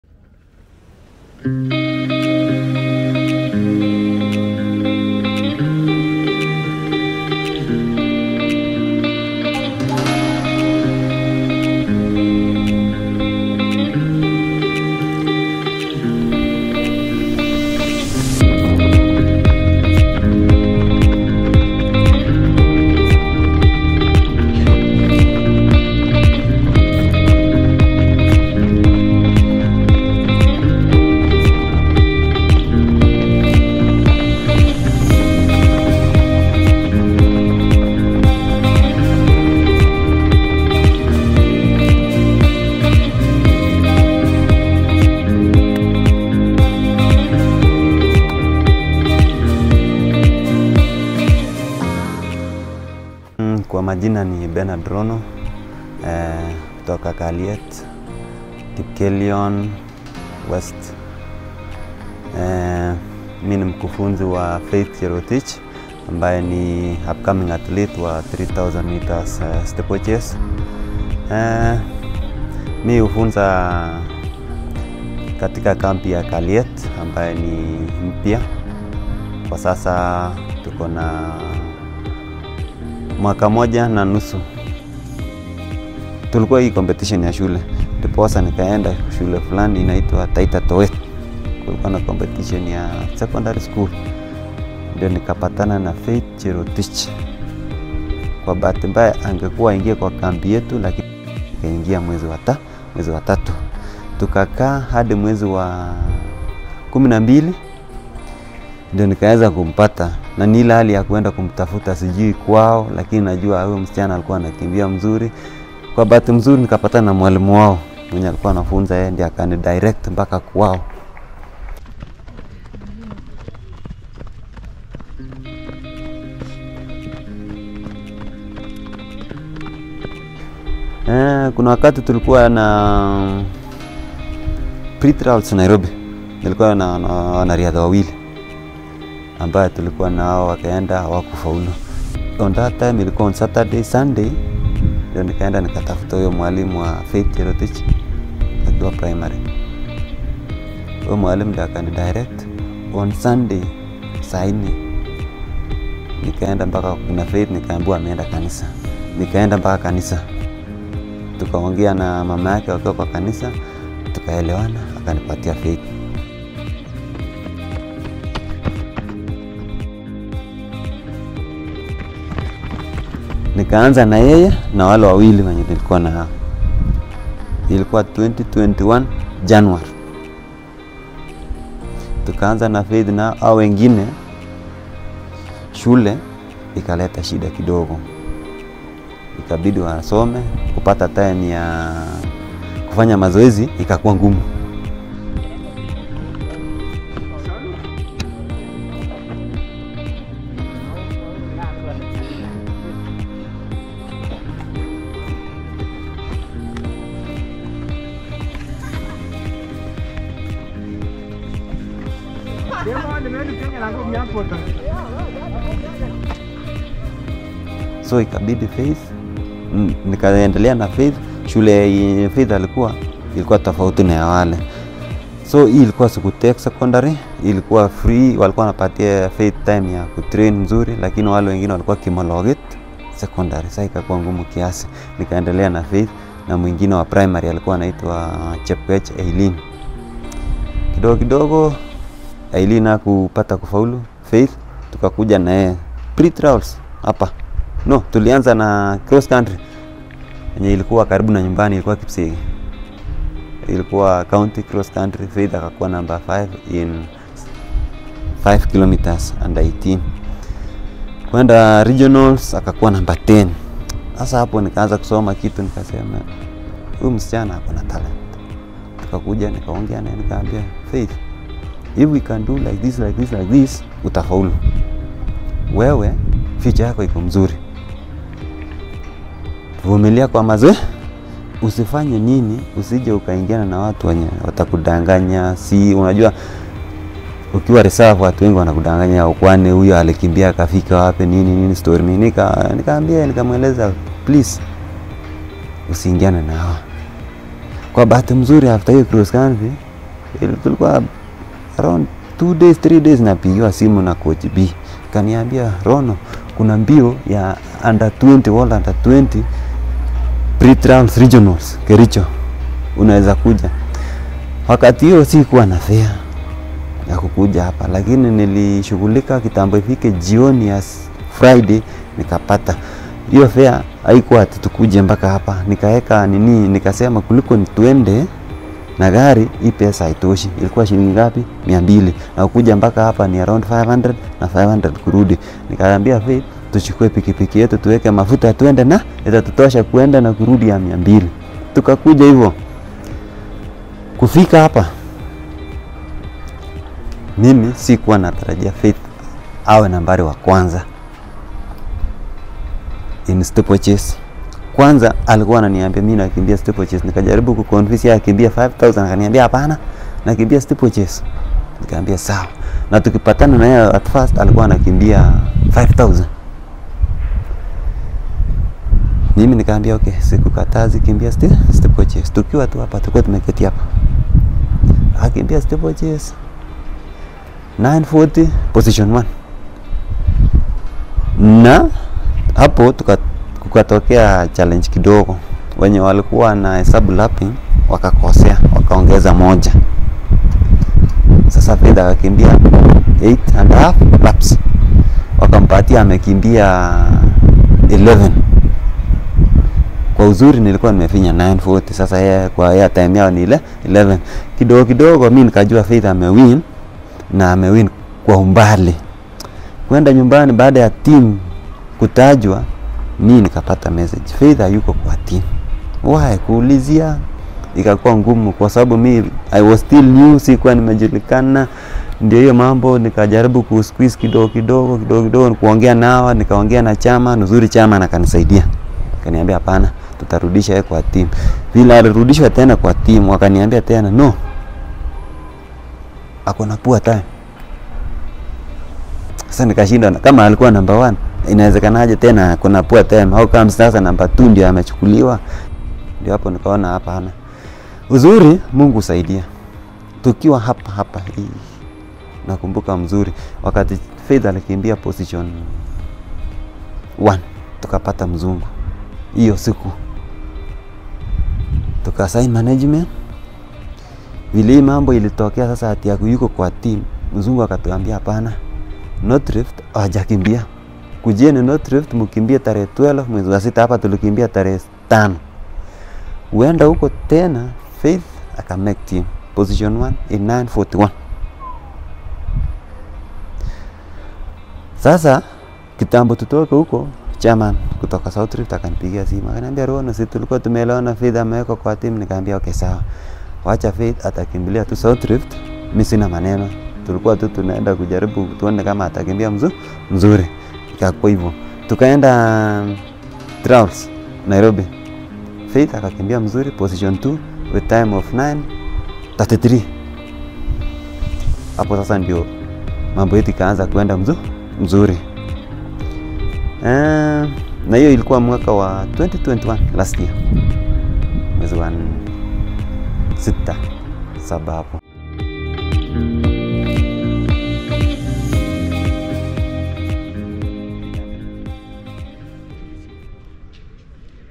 The Incredible Story of Faith Cherotich
Faith Cherotich is an upcoming track and field athlete, she hails from Kalyet, Kipkelion Constituency in Kericho County. Faith runs the Steeplechase and the 5000m Women track race under the Juniors category.
In this feature, we talk to Benard Rono, Faith's coach. He opens up about her incredible talent and the ups and downs that upcoming athletes usually face especially just before they start to shine.
Faith ranks highly among the best prospects in the 5000m and 3000m Women's track races from Kericho County.